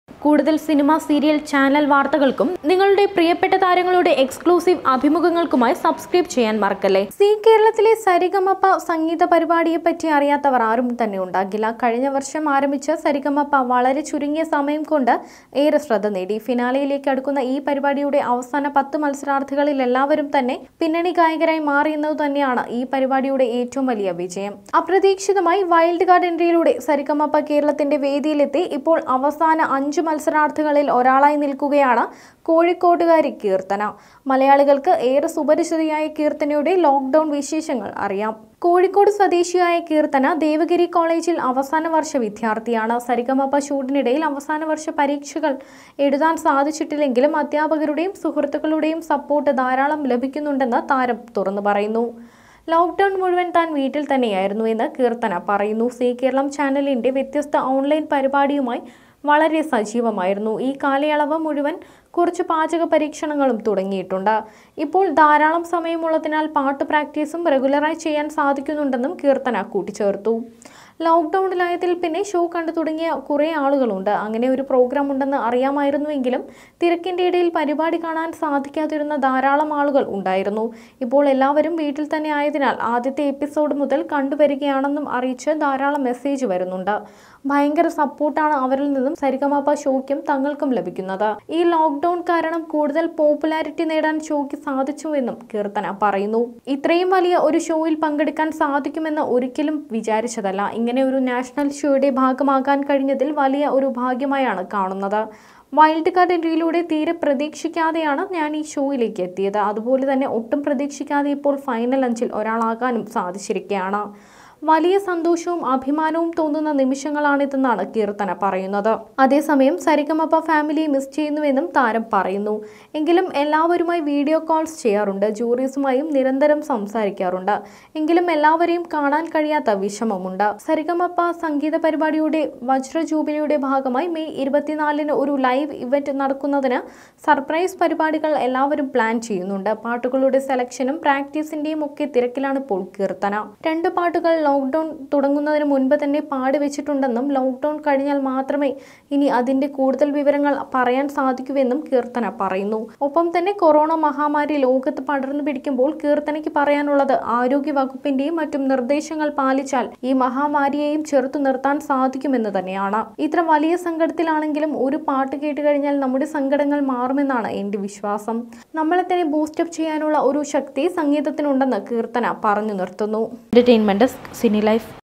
സരിഗമപ റിയാലിറ്റി ഷോയിലെ കീര്‍ത്തനയുമായി അഭിമുഖം..! l Keerthana
സീ കേരളത്തിലെ സരിഗമപ സംഗീതപരിപാടിയെപ്പറ്റി അറിയാത്തവര്‍ ആരും തന്നെ ഉണ്ടാകില്ല. കഴിഞ്ഞവര്‍ഷം ആരംഭിച്ച സരിഗമപ വളരെ ചുരുങ്ങിയ സമയംകൊണ്ട് ഏറെ ശ്രദ്ധനേടി. ഫിനാലെയിലേക്കടുക്കുന്ന ഈ പരിപാടിയുടെ അവസാന 10 മത്സരാര്‍ഥികളെല്ലാരും തന്നെ പിന്നണിഗായകരായി മാറിയെന്നതു തന്നെയാണ് ഈപരിപാടിയുടെ ഏറ്റവും വലിയ വിജയം. അപ്രതീക്ഷിതമായി വൈല്‍ഡ്കാര്‍ഡ് എന്‍ട്രിയിലൂടെ സരിഗമപകേരളത്തിന്റെ വേദിയിലെത്തി ഇപ്പോള്‍ അവസാന 5 മത്സരാര്‍ഥികളില്‍ ഒരാളായി നില്‍ക്കുകയാണ് കോഴിക്കോട്ടുകാരി കീര്‍ത്തന. മലയാളികള്‍ക്കു ഏറെ സുപരിചിതയായ കീര്‍ത്തനയുടെ ലോക്ഡൗണ്‍ വിശേഷങ്ങള്‍ അറിയാം.